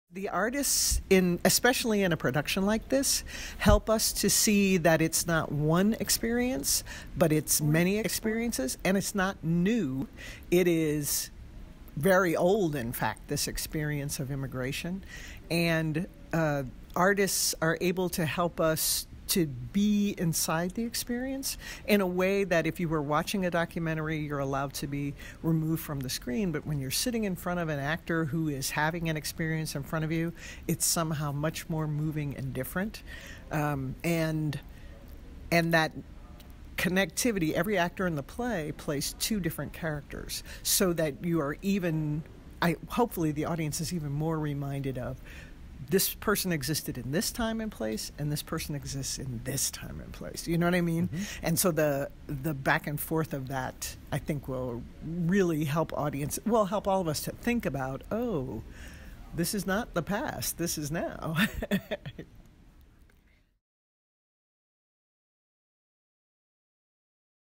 "Rowing to America: The Immigrant Project" at Cal State San Bernardino
“Rowing to America: The Immigrant Project” is a Department of Theatre Arts collection of nine one-act plays on the immigration, emigration and migration experience at California State University, San Bernardino.

In this short video, hear director Kathryn Ervin discuss why it's vital to explore challenging topics through art.